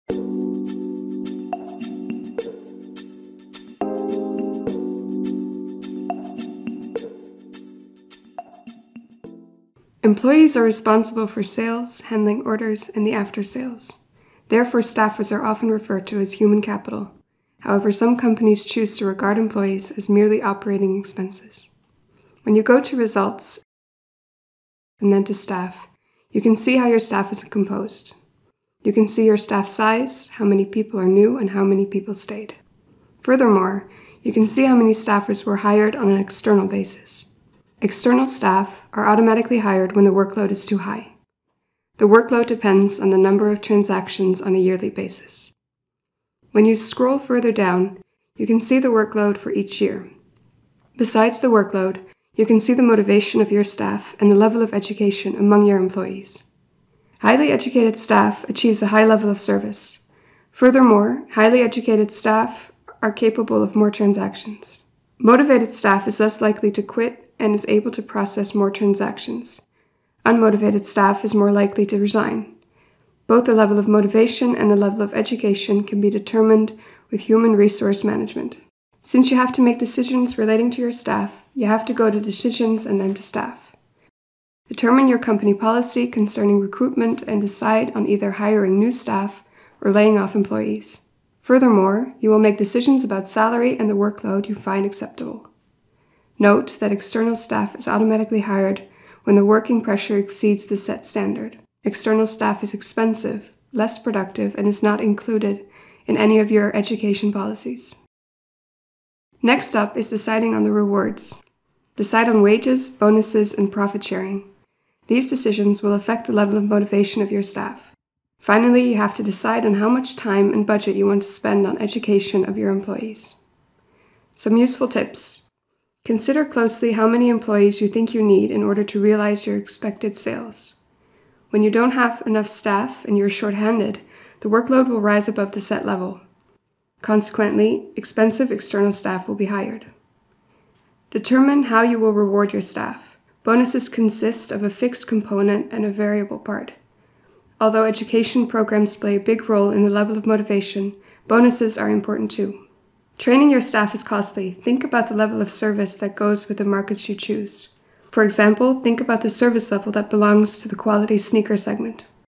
Sneakers Incorporated HR policy (5 of 6)
Instruction video of Sneakers Incorporated that explains the decisions within the HR department and the effect